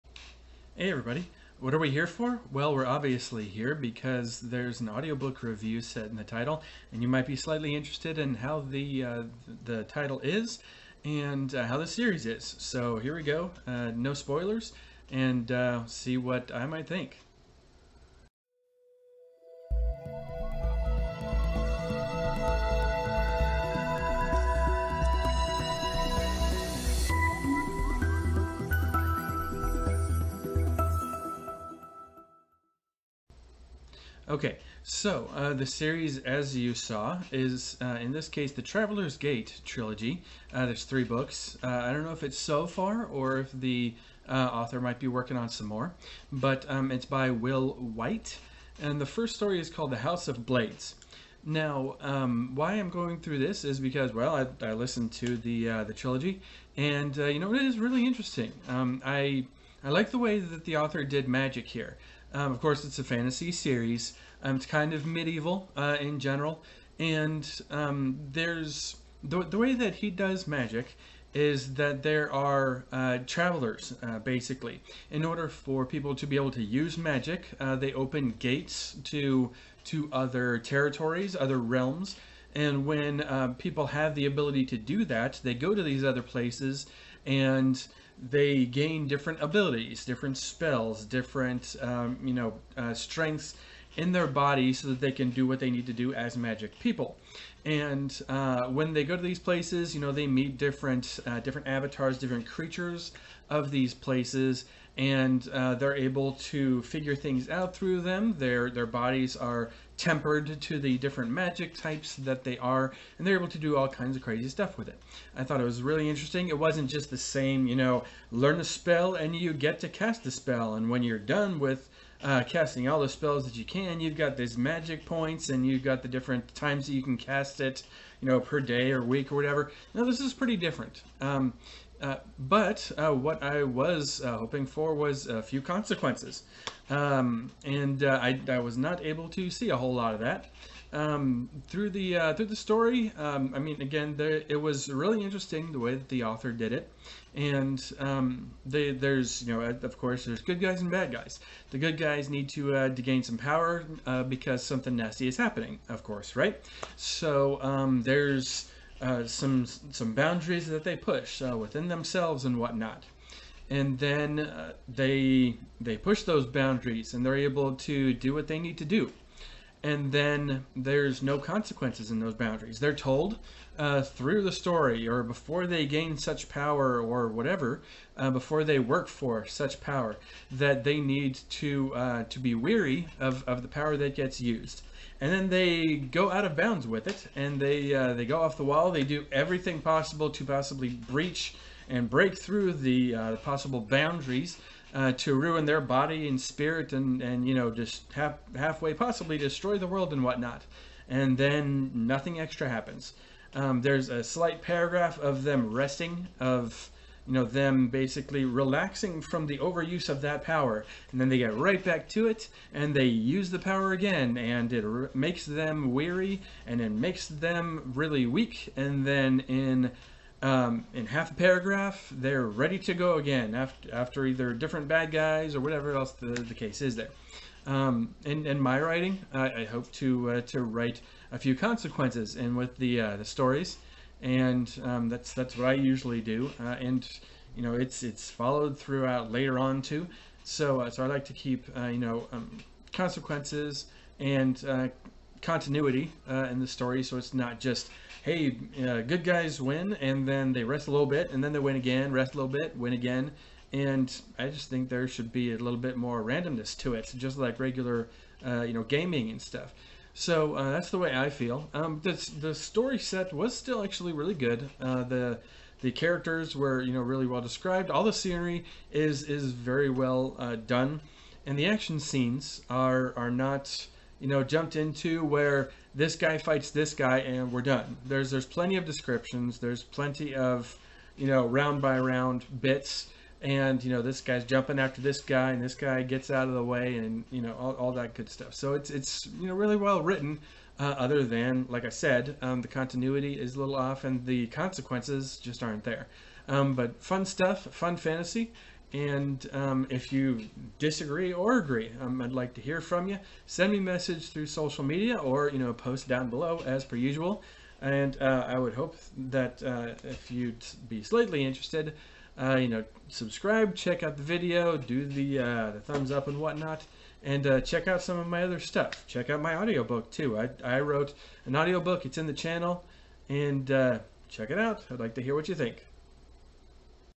Short review booktube litrpg - Travelers gate series - what do I think?
Fantasy series, what might I think?
Midevil setting, gloomy start, a different type of magic set, how did Will Wight do?